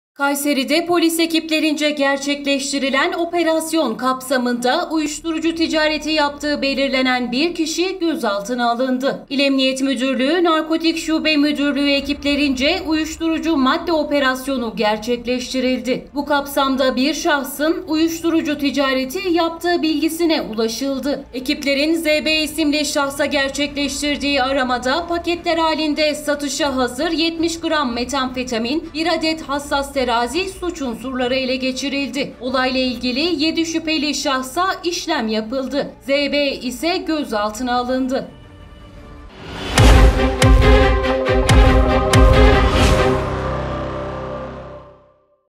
Kayseri’de Uyuşturucu Operasyonu: 1 Gözaltı
KAYSERİ,(BGC) - Kayseri’de polis ekiplerince gerçekleştirilen operasyon kapsamında uyuşturucu ticareti yaptığı belirlenen 1 kişi gözaltına alındı.

İl Emniyet Müdürlüğü Narkotik Şube Müdürlüğü ekiplerince uyuşturucu madde operasyonu gerçekleştirildi. Bu kapsamda Z.B isimli şahsın uyuşturucu ticareti yaptığı bilgisine ulaşıldı. Ekiplerin Z.B isimli şahsa gerçekleştirdiği aramada paketler halinde satışa hazır 70 gram metamfetamin, 1 adet hassas terazi suç unsurları ele geçirildi. Olay ile ilgili 7 şüpheli şahsa işlem yapılarak, Z.B isimli şahıs ise gözaltına alındı.